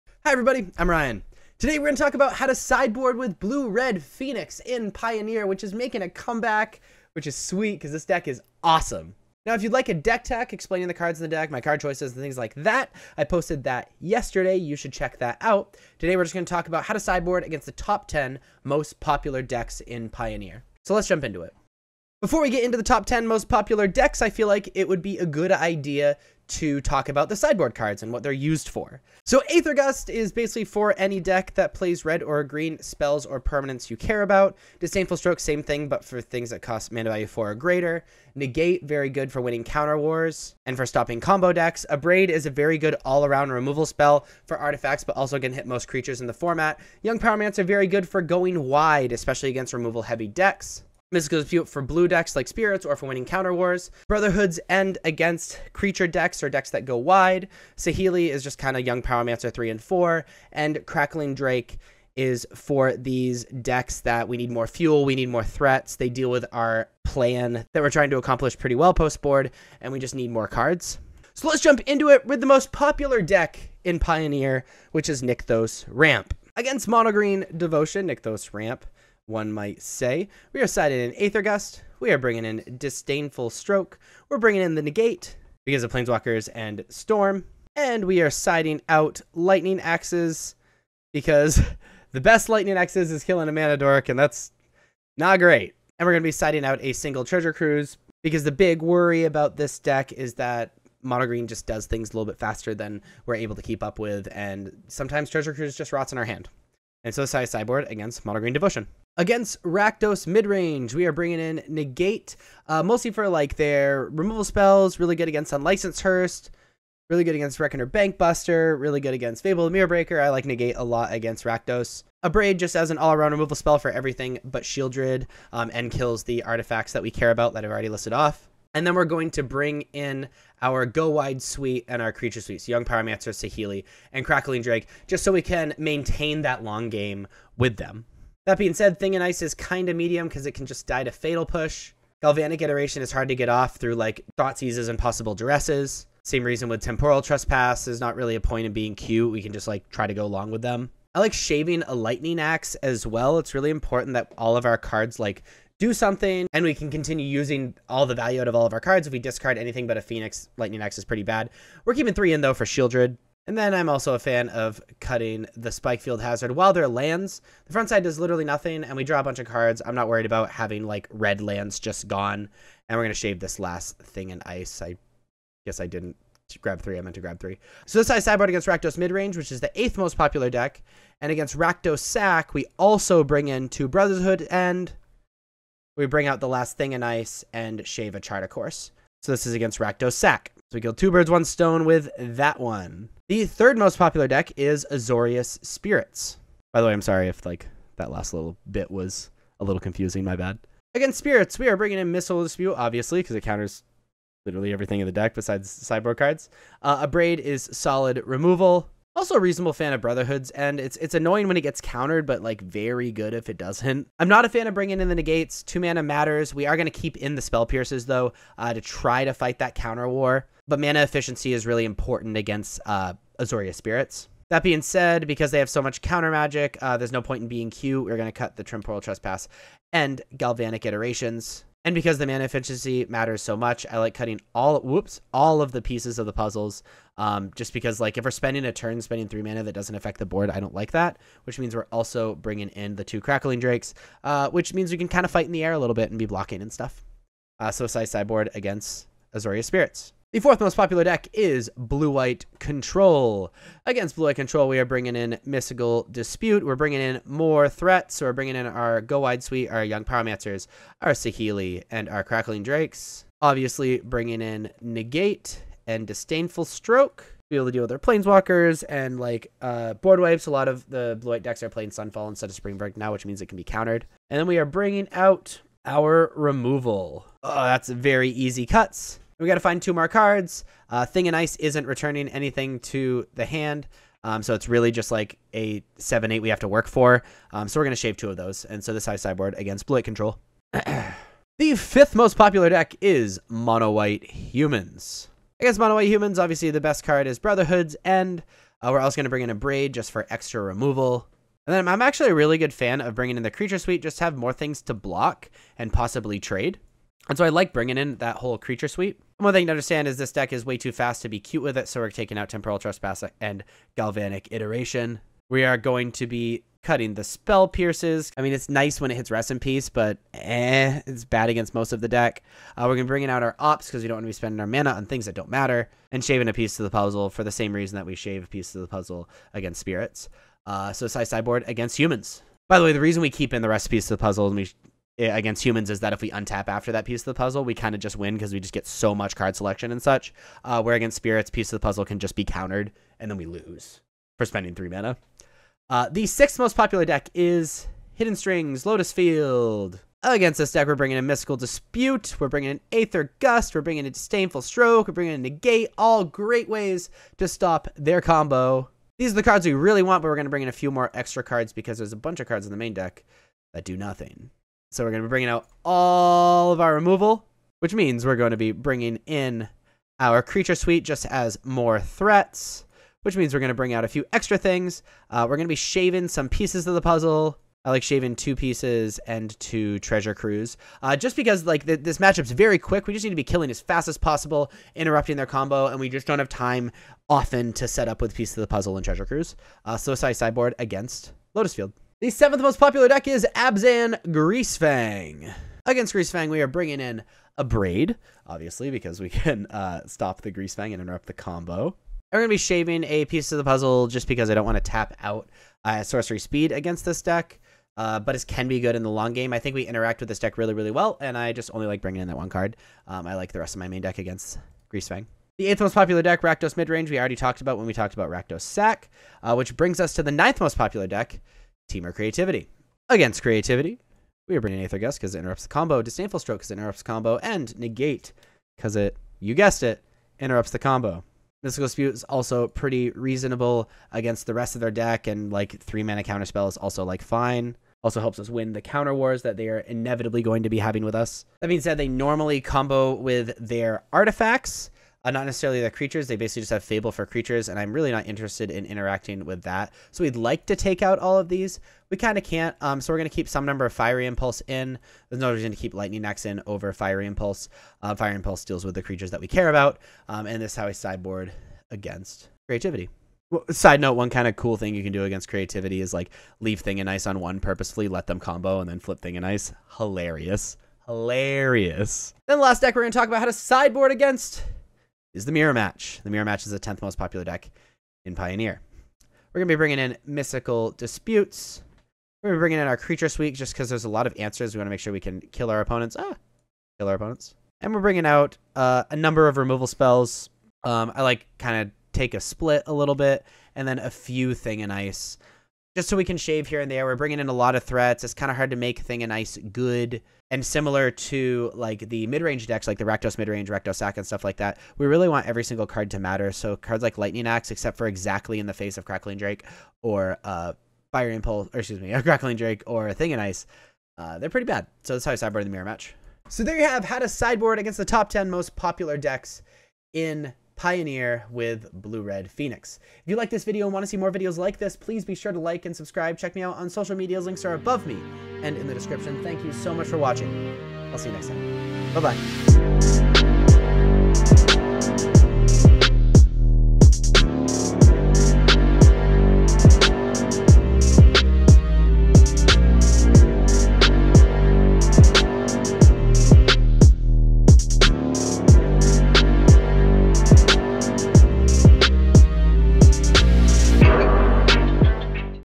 [PIONEER] UR Phoenix | Sideboard Guide
Click▼

UR Phoenix has fallen off as of late, however, it is still a powerful force and may just be a way to break the meta wide open.  In this sideboard guide I am going to go over why we play cards in the sideboard and how to board against the top 10 most popular decks in Pioneer.